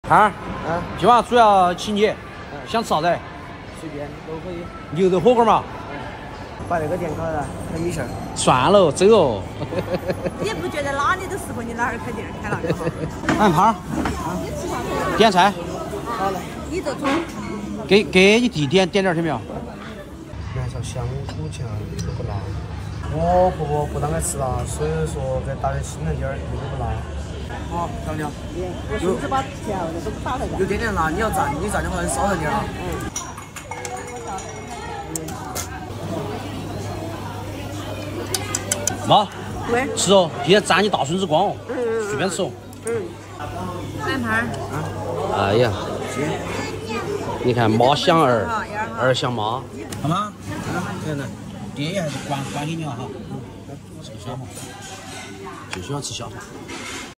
请胖胖去吃火锅，胖胖全程照顾团团太暖了！
哈喽
这里是杨翠花，杨翠花的杨，杨翠花的翠花！🌸
直爽的四川辣妹子！爱哭、爱闹、爱笑、爱交朋友😀

家有“两厨”，超小厨和我的小小厨。